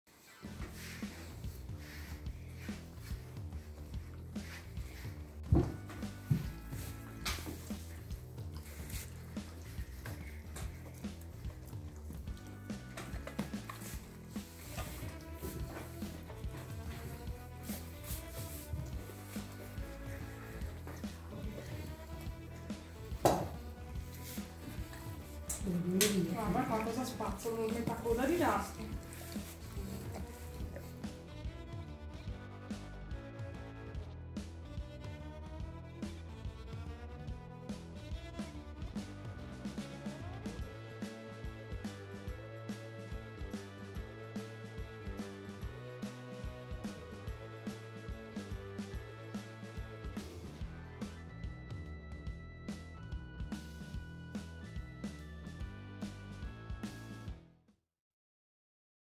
BRUSHING MY DOGS' TEETH! Funny Bernese Mountain dog reaction
One of the duties of  dog owners is taking care of their dogs. When you have Bernese Mountain dogs, it's important to brush their teeth regurarly!
And you, do you brush your dog's teeth?